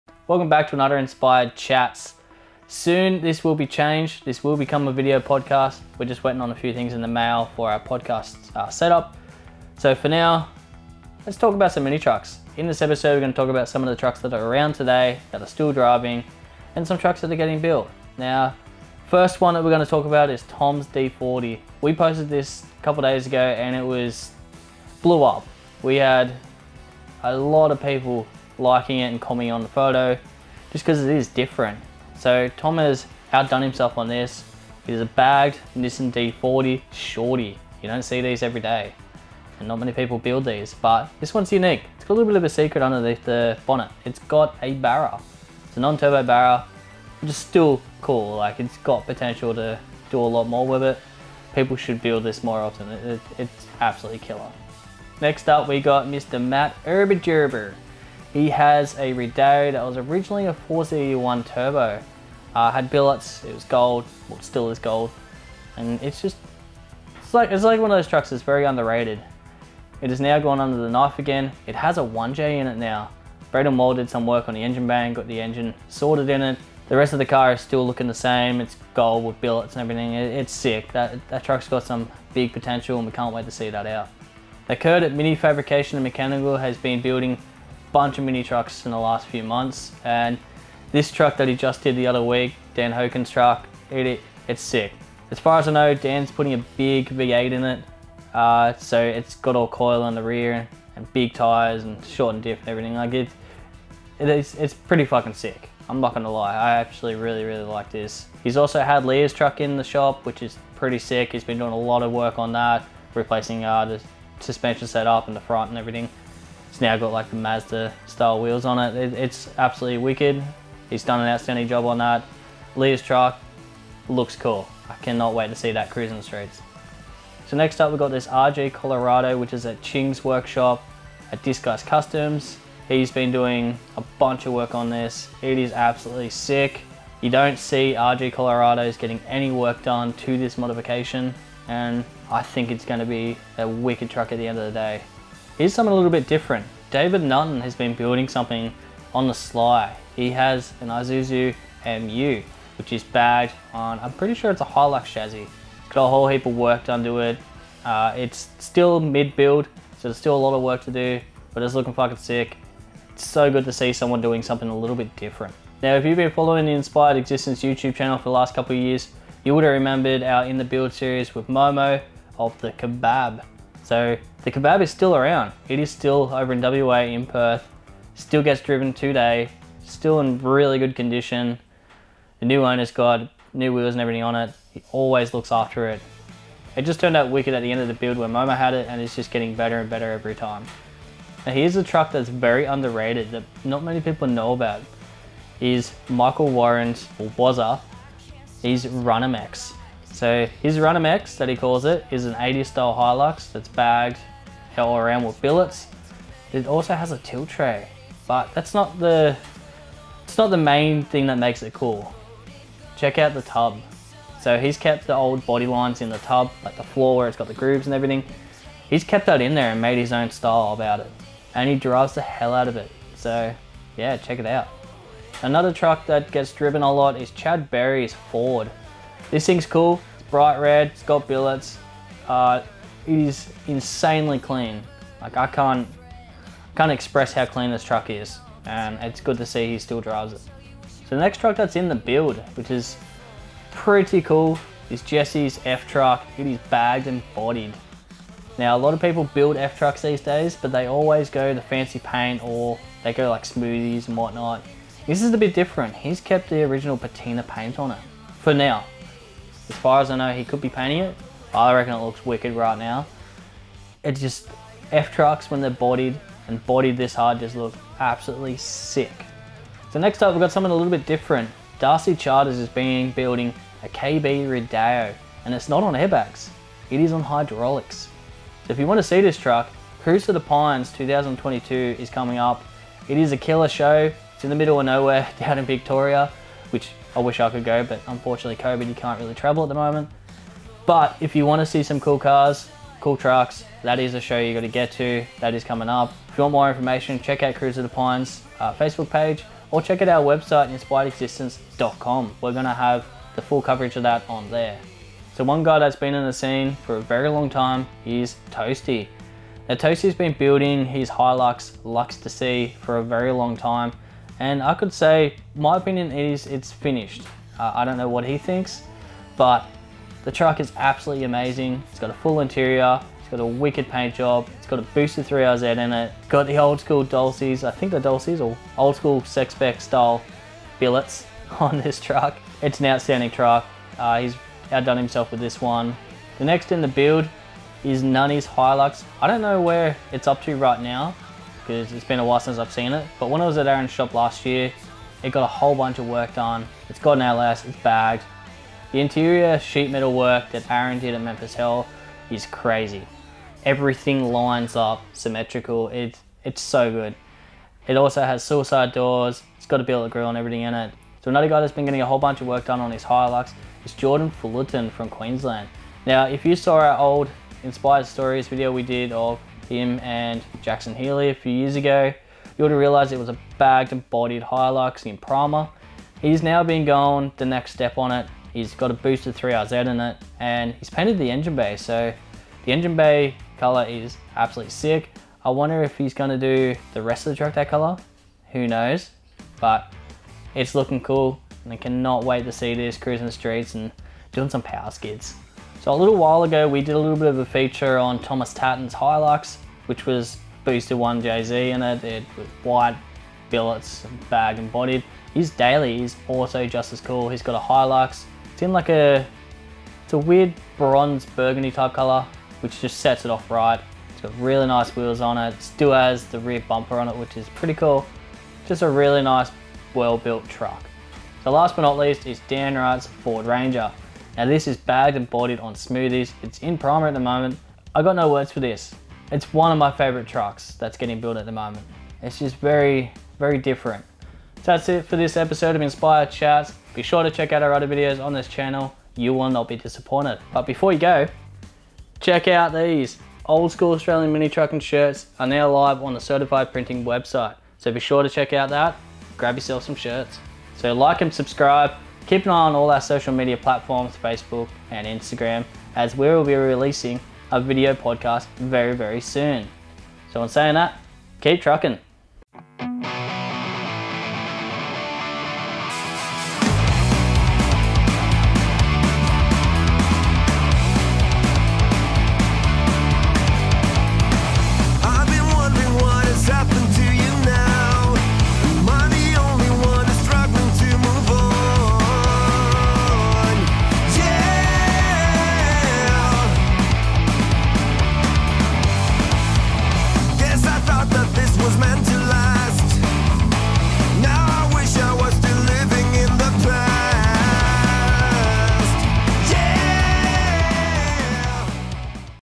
Minitrucks around today - InspiredCHATS
In this episode we talk about some trucks that are the build and some trucks that are driving around today.
FOR YOUR MINITRUCKIN MERCHANDISE.